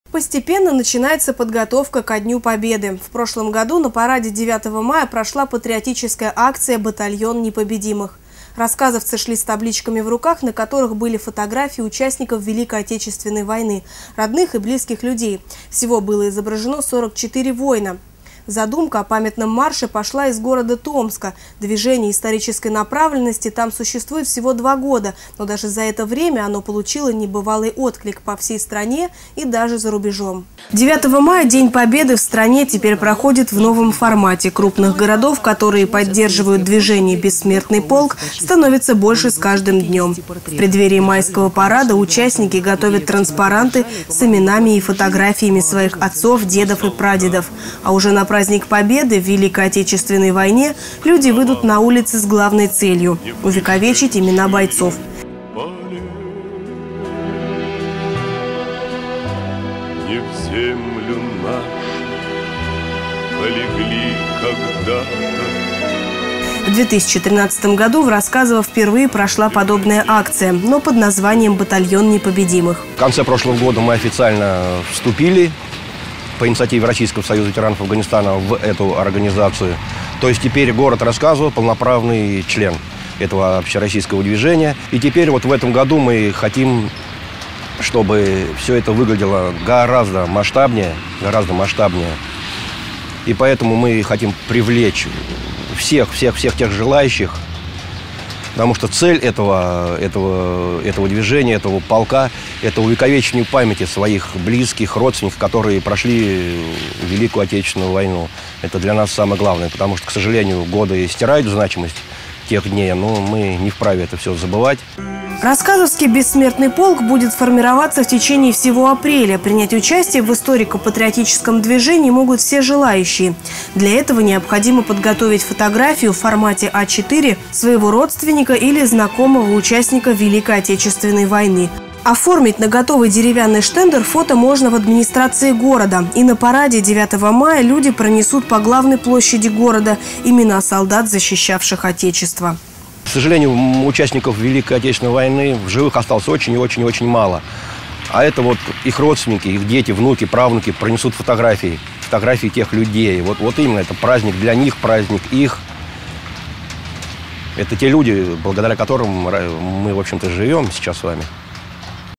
«Бессмертный полк» пройдет по площади Рассказово 9 мая
Постепенно начинается подготовка ко дню Победы. В прошлом году на параде 9 мая прошла патриотическая акция «Батальон непобедимых». Рассказовцы шли с табличками в руках, на которых были фотографии участников Великой Отечественной войны, родных и близких людей. Всего было изображено 44 воина. Задумка о памятном марше пошла из города Томска. Движение исторической направленности там существует всего 2 года, но даже за это время оно получило небывалый отклик по всей стране и даже за рубежом.

Юлия Хлыстова